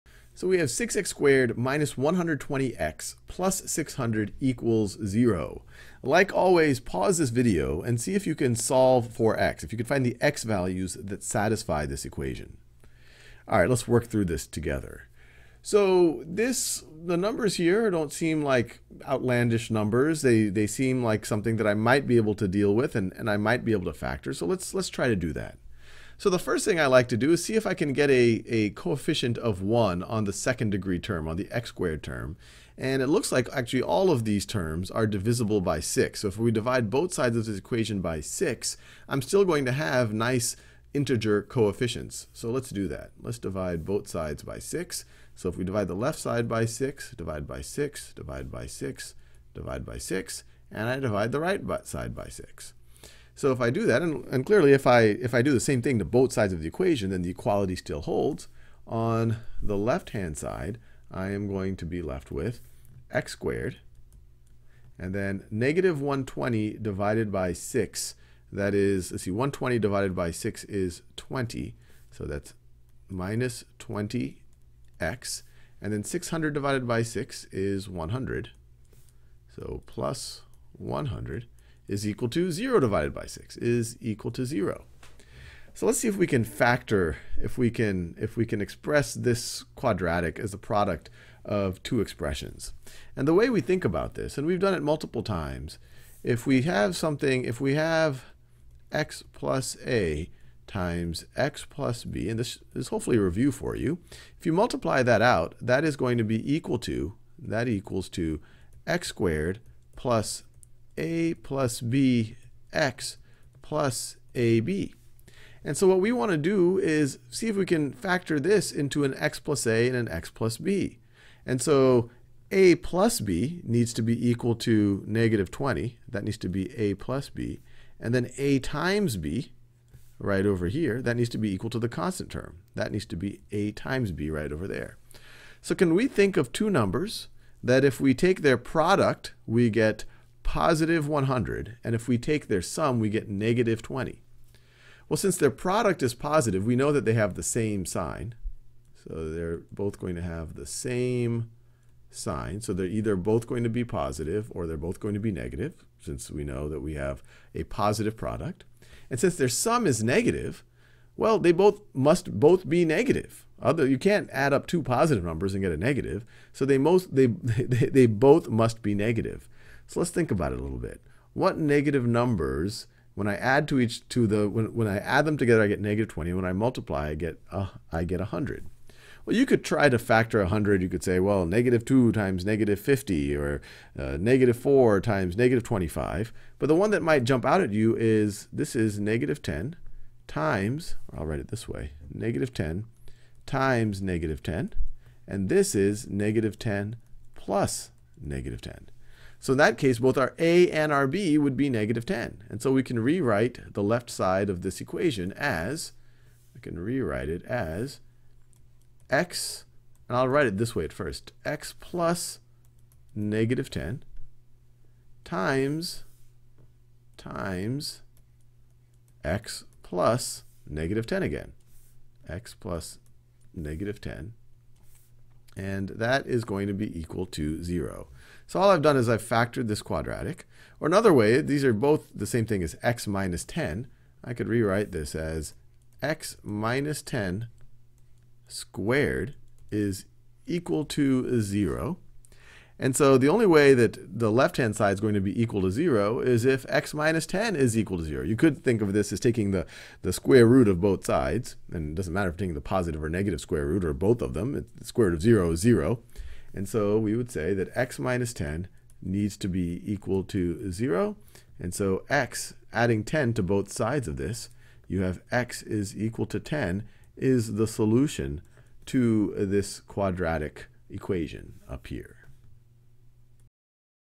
Solving quadratics by factoring: leading coefficient Ã¢Â Â 1 | High School Math | Khan Academy
Sal solves 6x²-120x+600=0 by first dividing by 6 and then factoring.